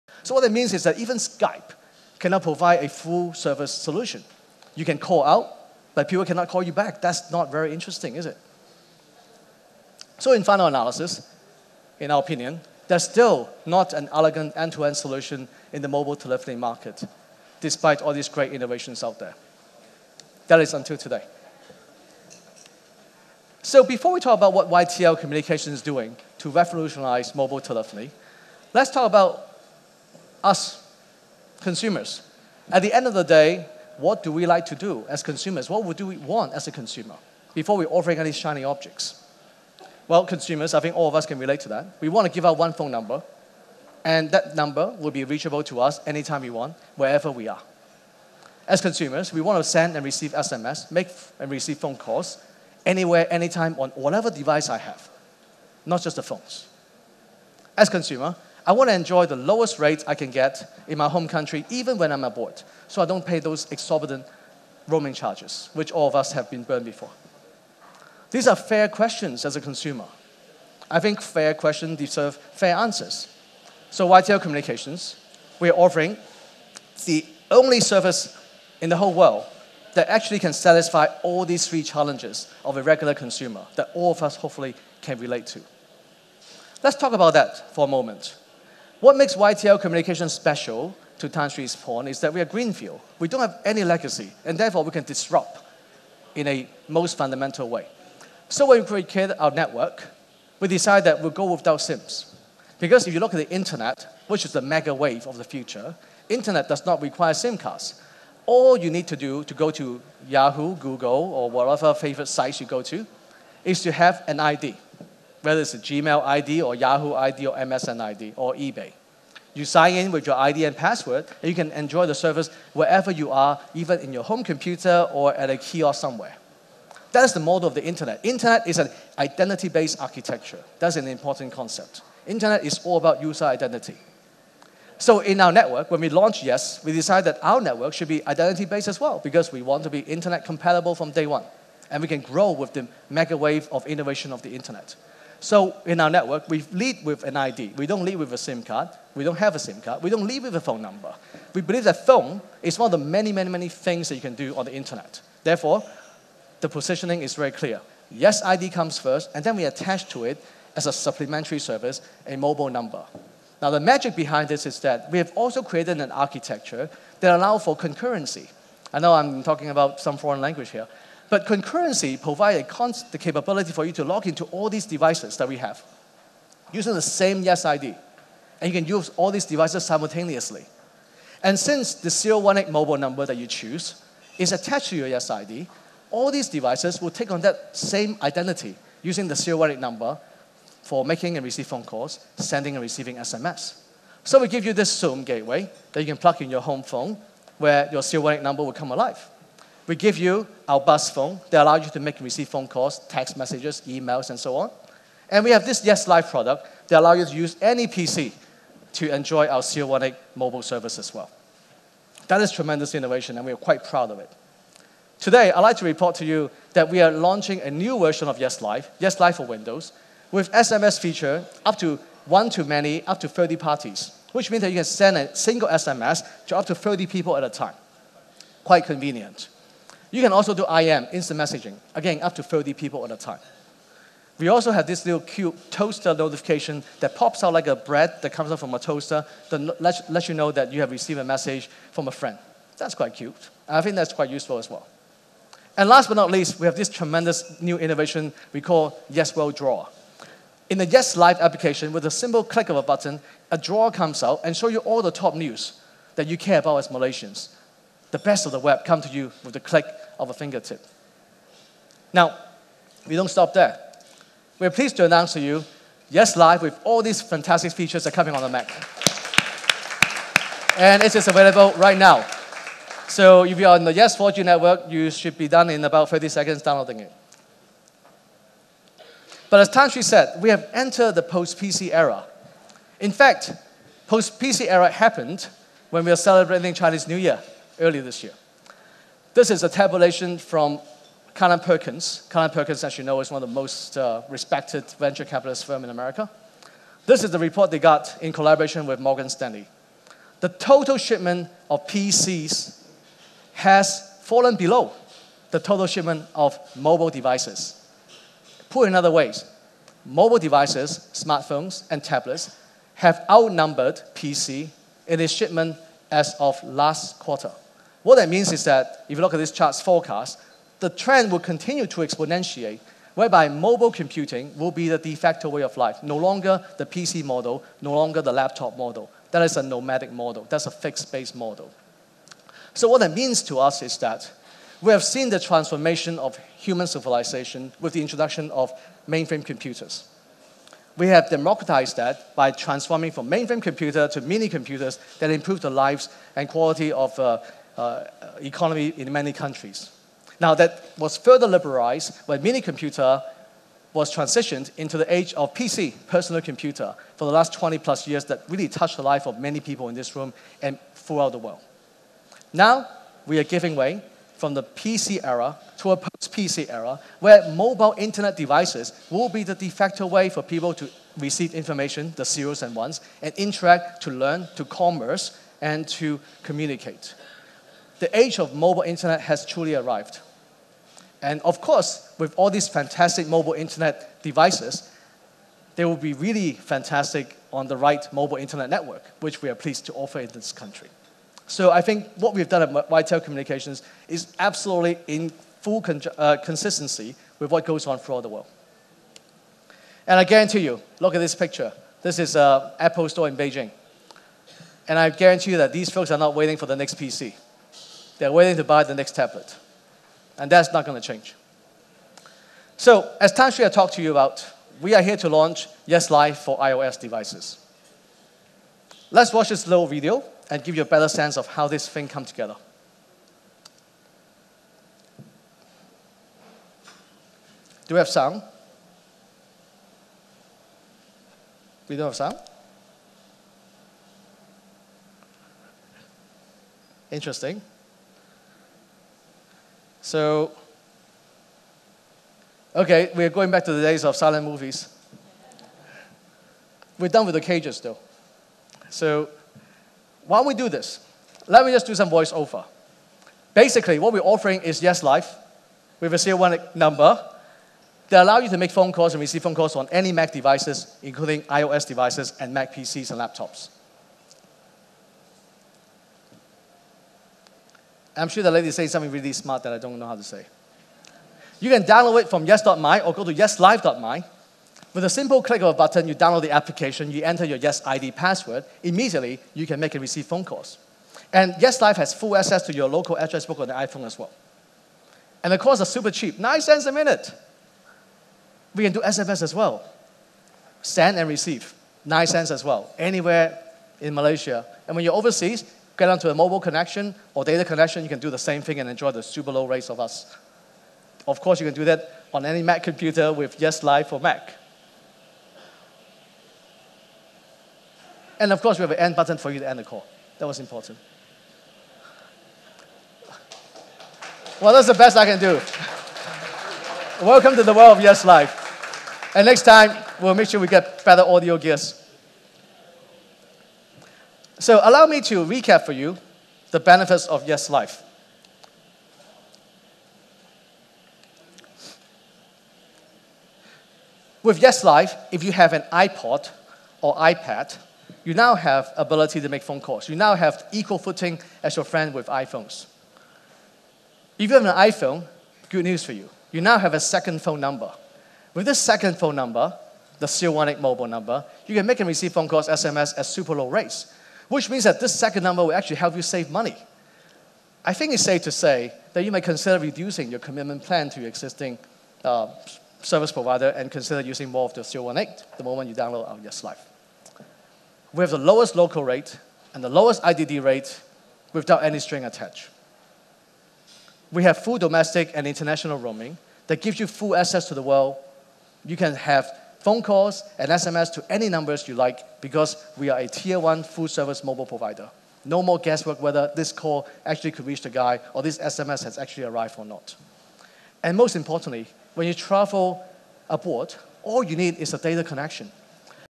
Wing K Lee: YES 4G Reinvents the Mobile Phone with Yes Life
Yes Life for iOS by YES 4G reinvents the mobile phone.